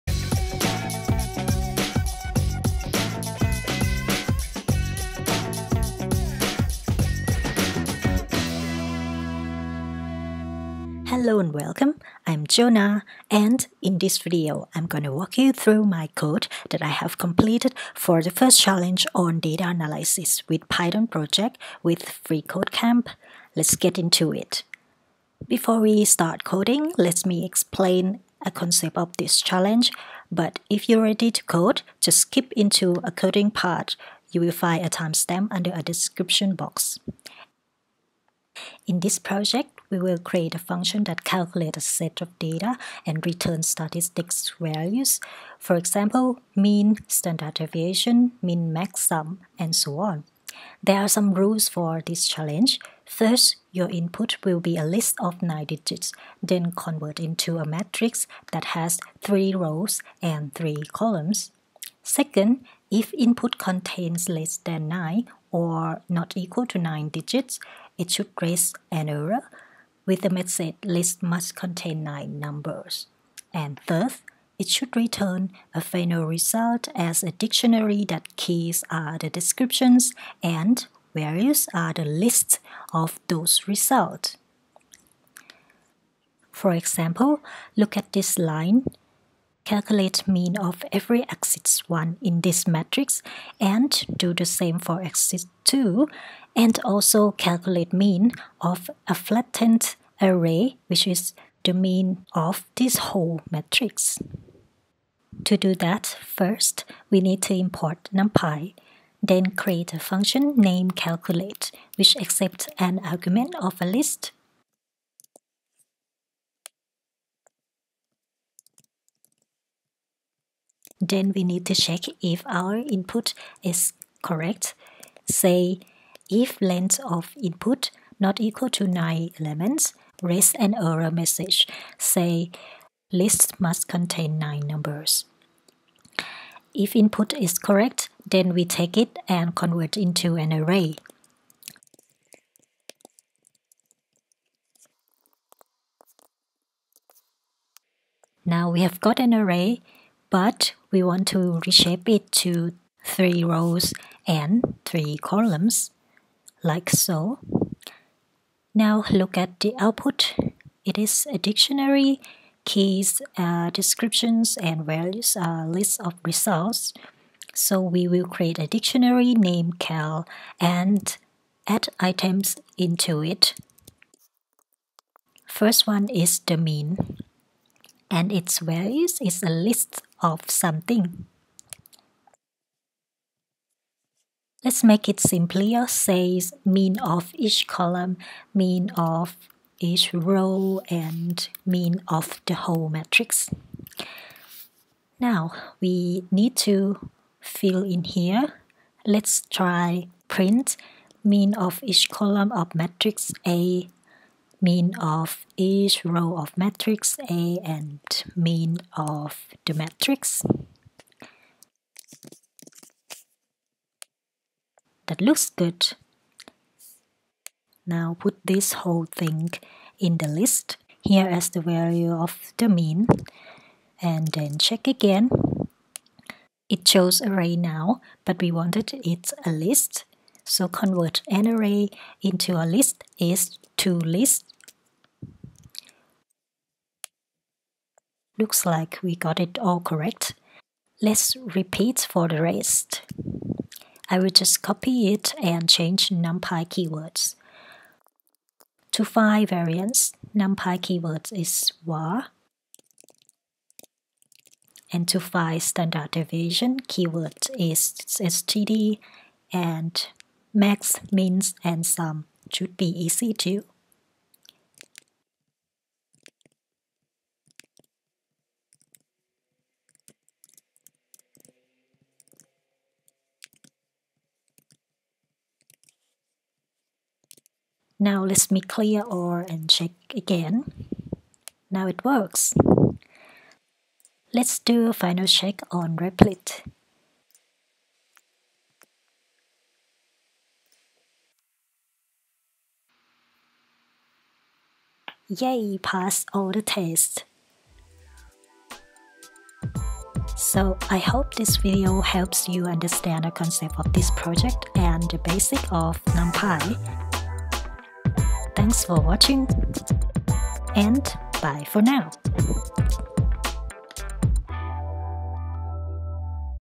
Mean-Variance-Standard Deviation Calculator | Python/Numpy Project| FreeCodeCamp
How to calculate Mean-Variance-Standard Deviation with Python/Numpy
1:44 - Start coding
0:39 - Project idea explanation/Understand the assignment

This is part of the projects on freeCodeCamp that I'm working on.
Any questions or any experiences to share about this project?
Which project do you want to see next?